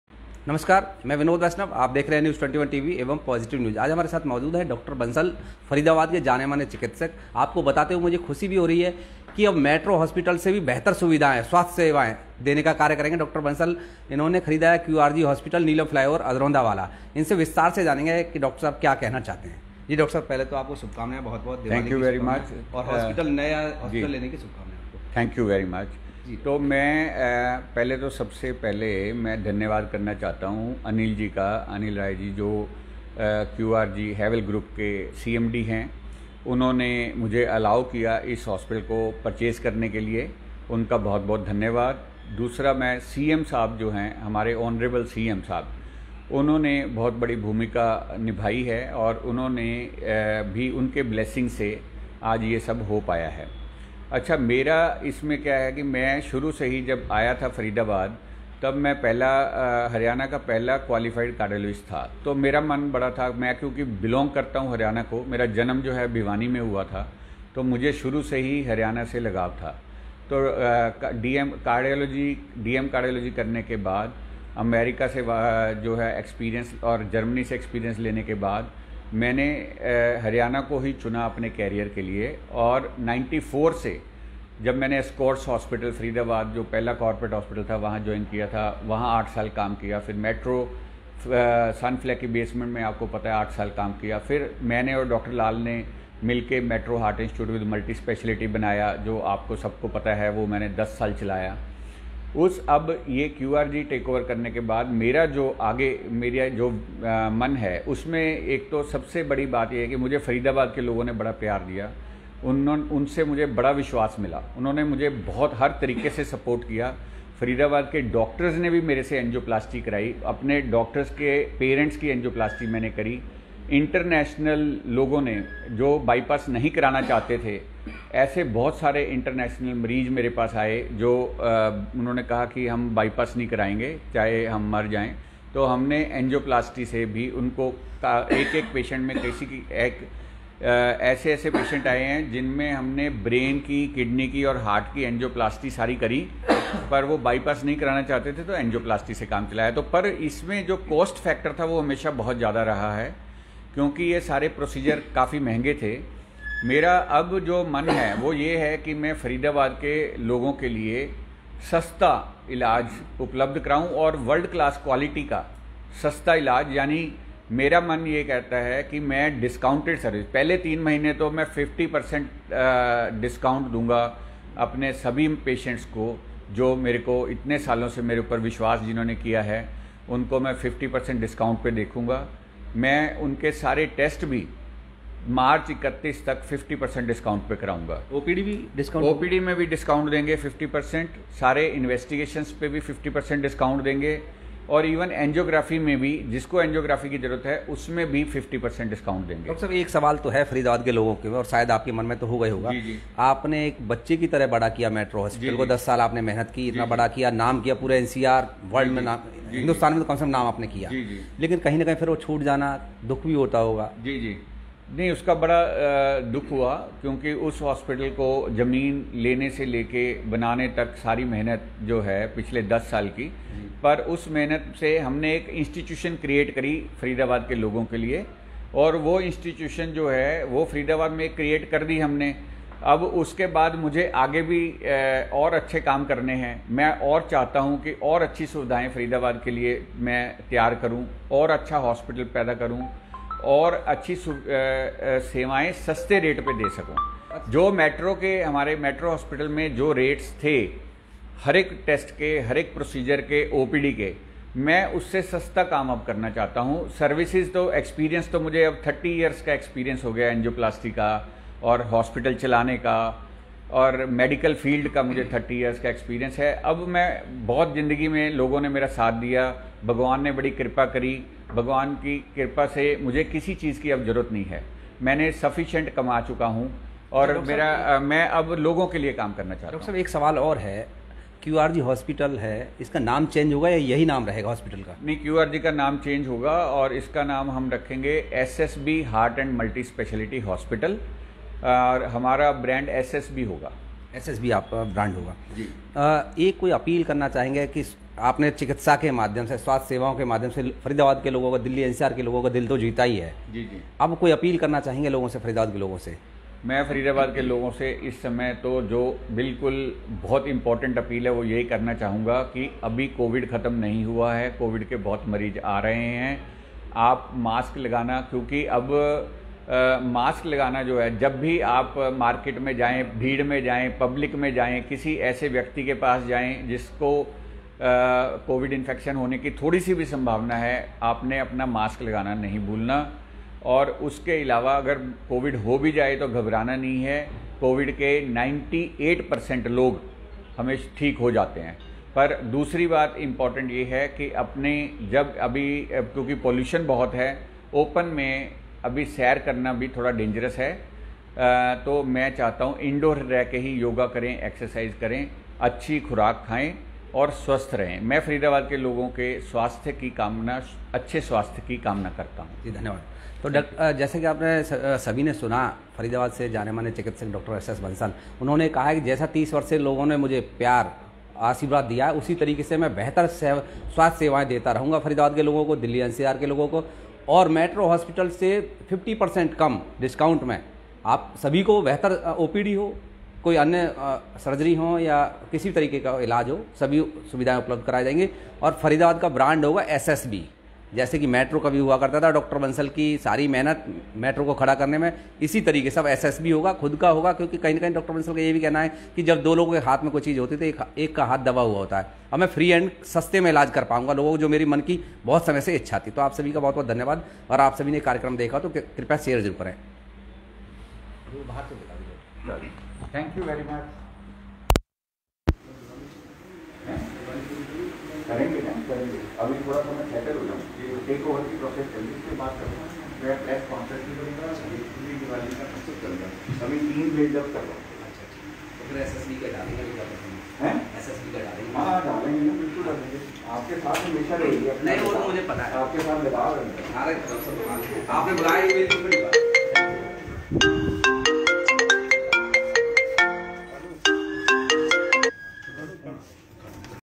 मेट्रोहॉस्पिटल छोड़ डॉ बंसल ने QRGhospital नीलमचौक वाला खरीद बेहतर स्वास्थ्य सेवा देने का वायदा किया
मेट्रो हॉस्पिटल को छोड़कर डॉ एस एस बंसल ने QRG hospital नीलम चौक वाला खरीदकर बेहतर स्वास्थ्य सेवा देने का वायदा किया